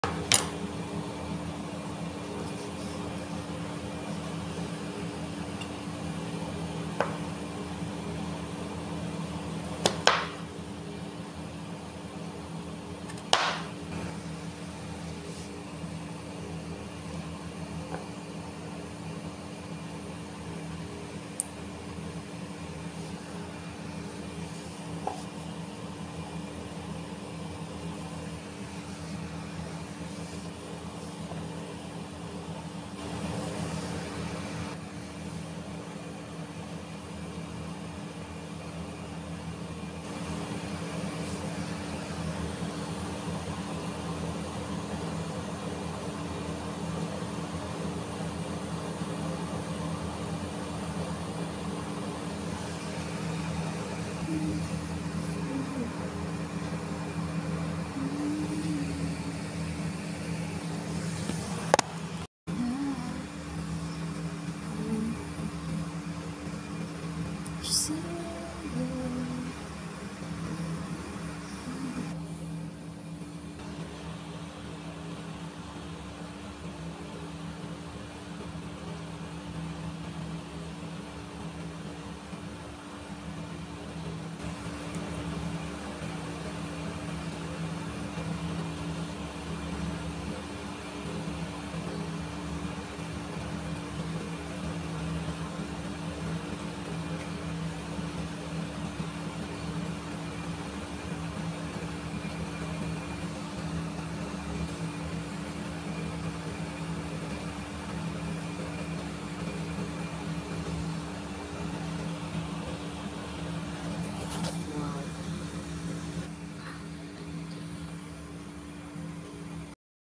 easy garnish tomato skin and carrots,papakita ko lang natutunan ko DTO sa youtube
this video is my own design hehehe
I used tomoto and carrots
 I'm not master to make this art garnish vegetables.